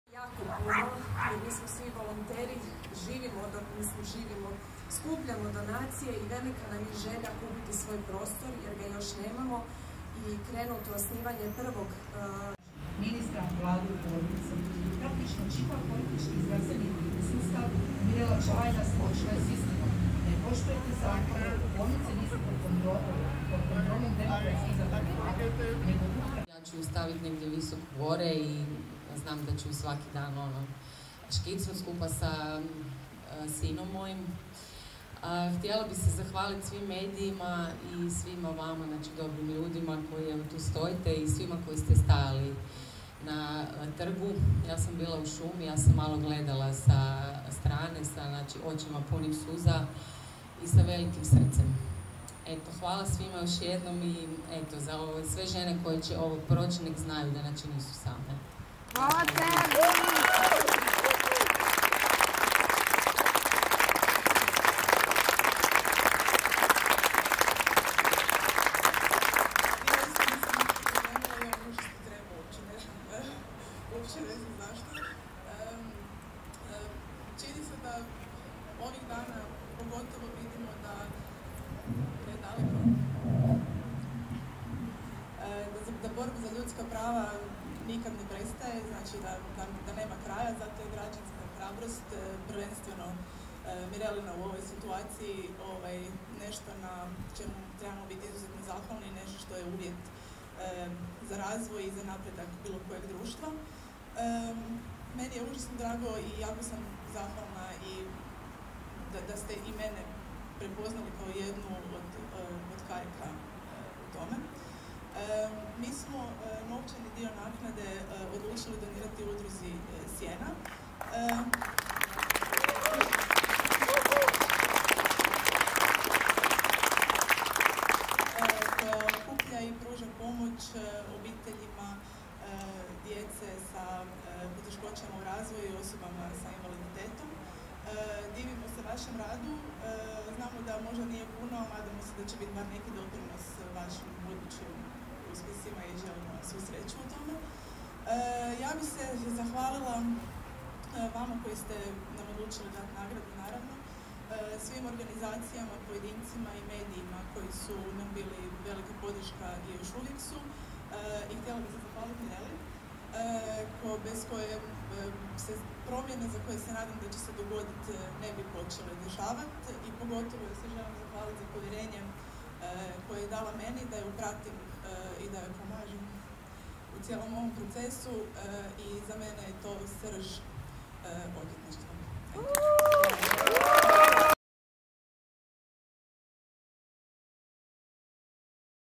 Mirela Čavajda i Vanja Jurić dobitnice su nagrade "Nada Dimić"
Ispred Hrvatskog društva likovnih umjetnika (HDLU) na Trgu žrtava fašizma dodijeljene su nagrade "Nada Dimić" i "Vesna Kesić". Dobitnici nagrade Vanja Jurić i Mirela Čavajda.